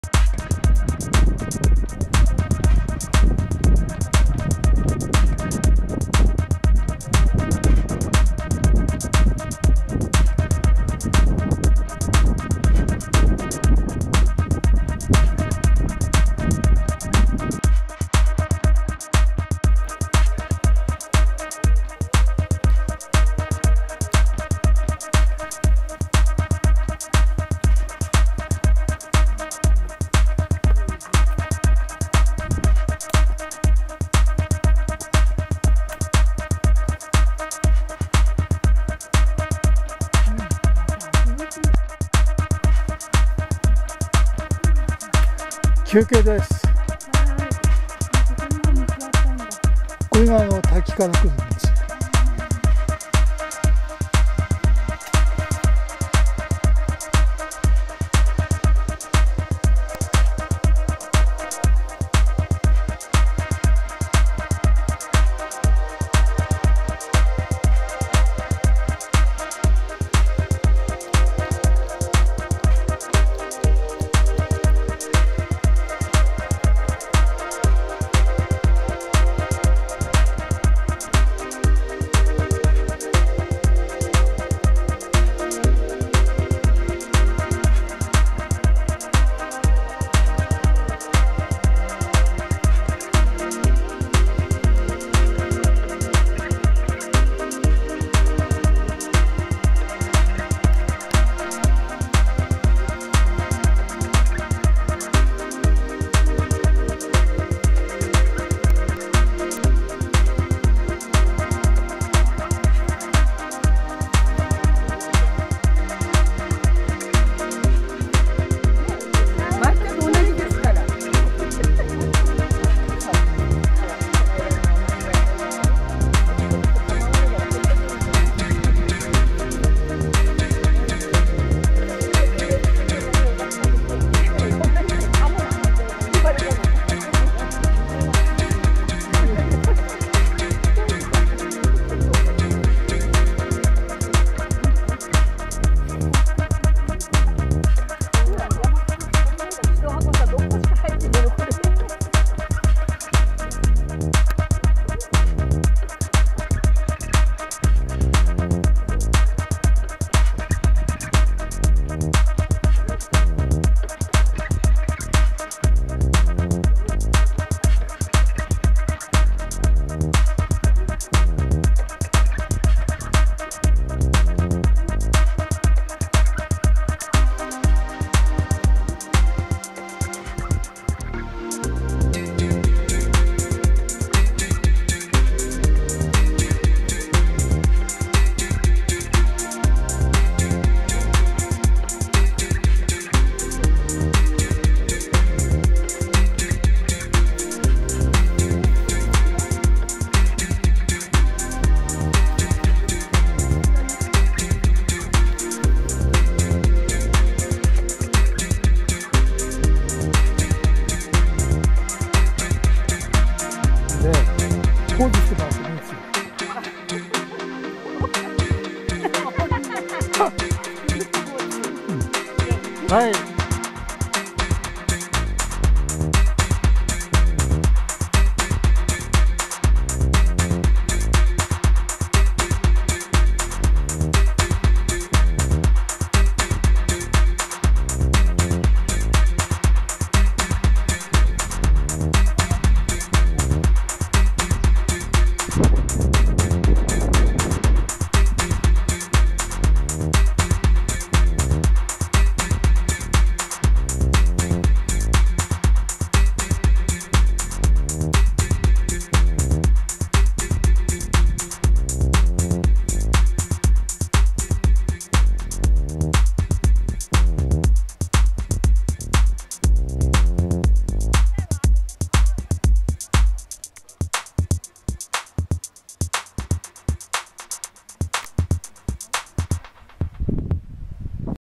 山形の初夏 ZAO新緑周遊トレック 2018.05.20
蔵王龍山を起点に中央高原へ縦走、ゲレンデを下降し蔵王温泉へと周遊トレッキング、萌える新緑と初夏の花々との出会いを楽しむ、、、